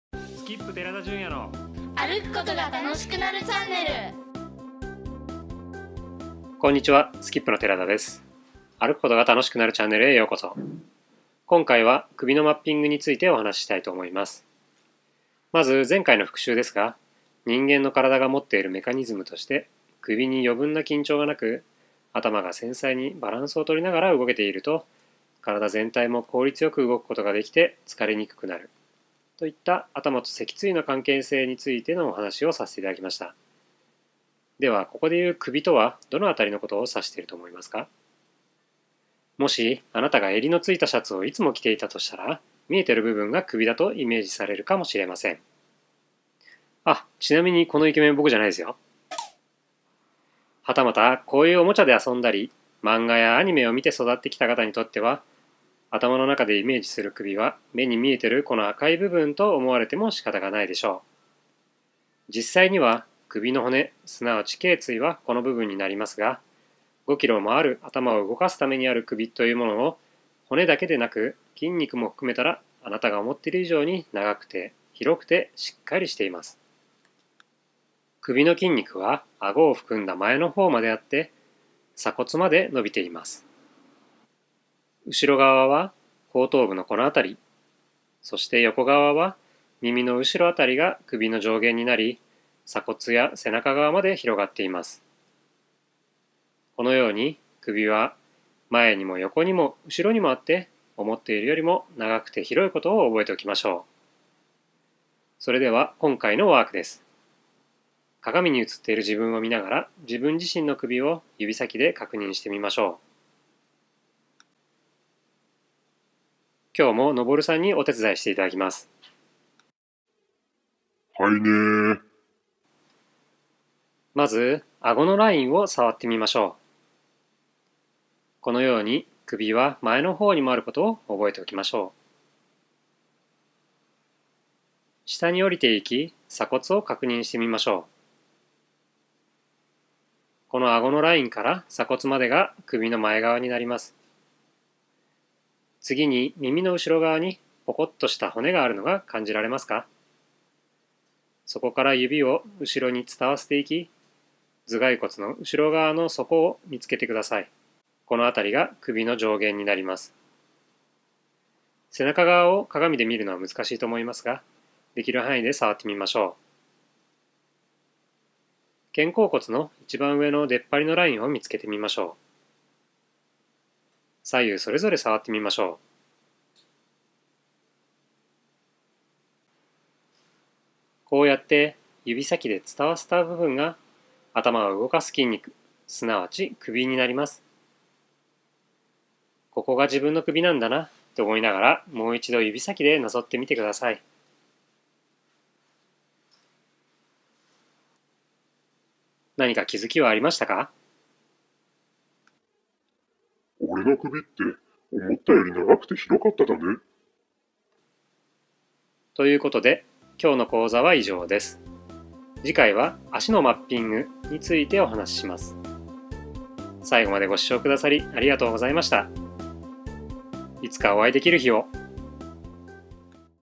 【#9 疲れない姿勢入門講座】〜 首のマッピング
こんにちは、SKiPの寺田です。

今回は、「首のマッピング」についてお話ししました。

頭が脊椎の上でバランスを取りながら動けていると、結果的に身体全体がラクになり、疲れや痛みのない生活を送ることができます。

そのためにも、首は思っている以上に長くて広くてしっかりとしていることを覚えておきましょう。

静岡県浜松市にある足と靴の専門店SKiP（スキップ）で代表を務めています。